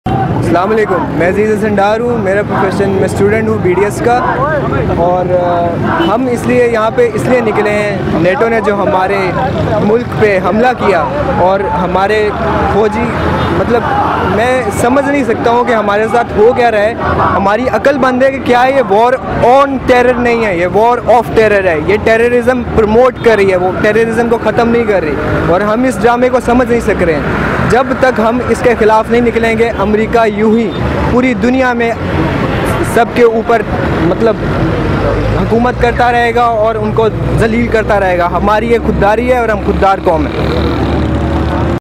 EProtest against NATO Attack - Aziz Hasan
Be a Part of World's First EProtest against NATO Attack on Pakistan. Upload your Recorded message with Title "EProtest against NATO Attack"